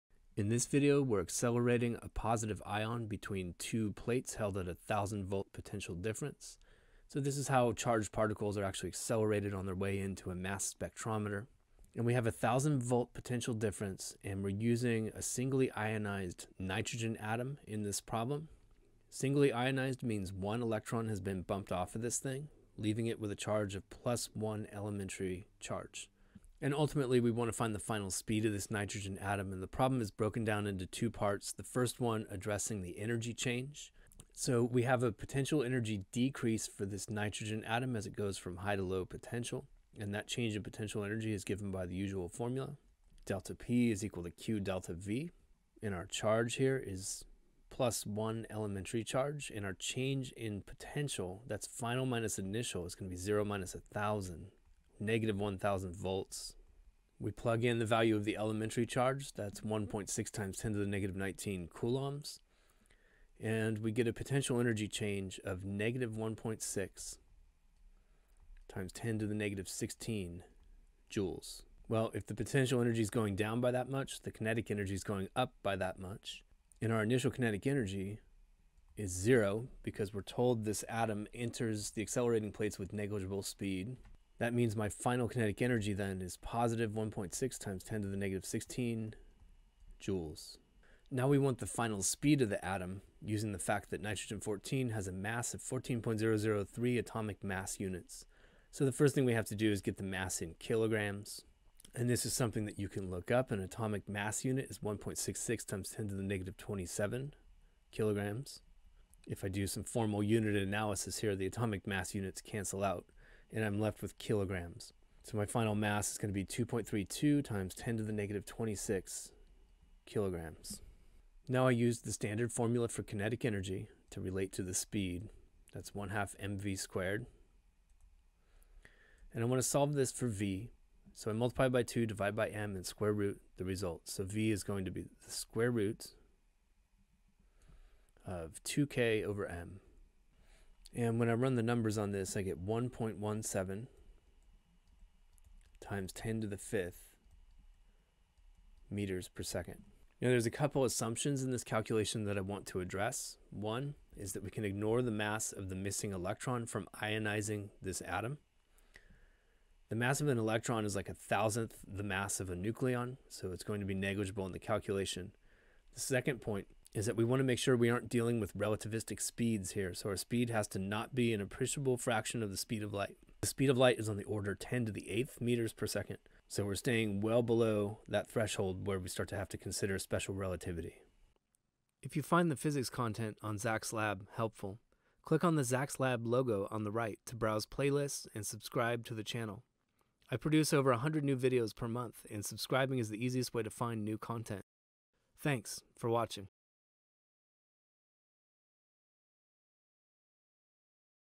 Accelerating plates Nitrogen ion. Charge accelerated through potential difference: energy and speed.
Accelerating plates Nitrogen ion accelerates through a potential difference of 1000V.

In this charge accelerated through potential difference problem, we start with a singly ionized Nitrogen atom accelerated through potential difference.  After the potential difference, energy and speed are calculated.

The kinetic energy of the ion is calculated by finding the drop in electric potential energy of the ion between the plates.  Once we have the kinetic energy, we apply the kinetic energy formula to find the speed of the ion when it passes through the ground plate.